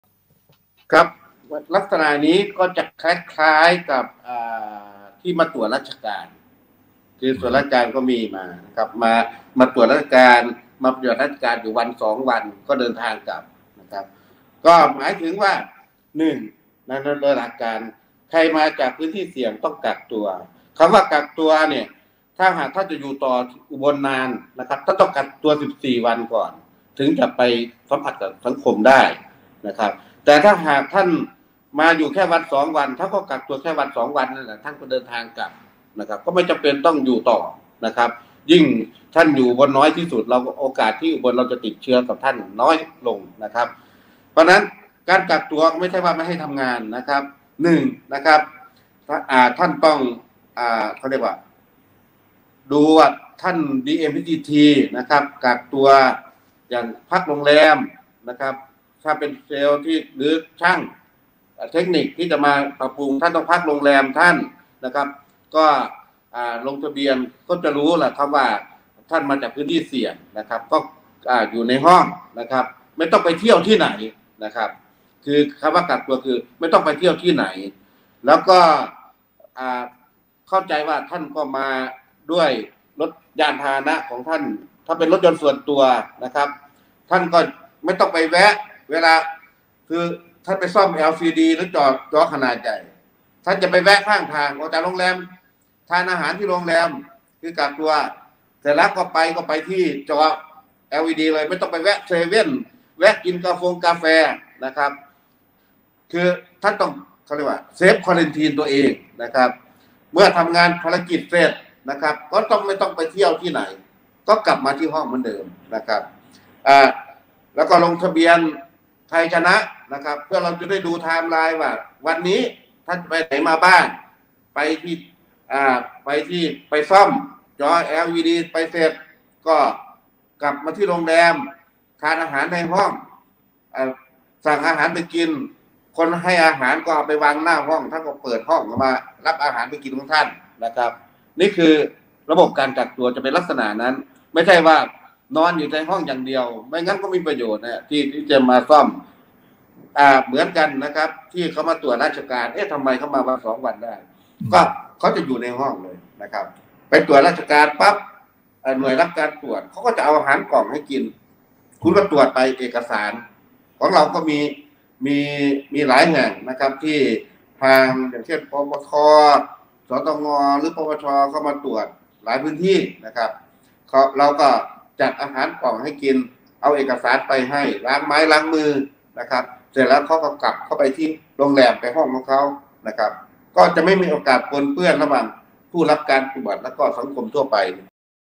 เซลล์แมน ช่าง เข้าอุบลฯ ควรปฏิบัติตัวอย่างไร?
เกริกชัย ผ่องแผ้ว รองผู้ว่าราชการจ.อุบลราชธานี
บางส่วนจากการแถลงข่าว สถานการณ์โควิด19อุบลฯ
25 มิถุนายน 2564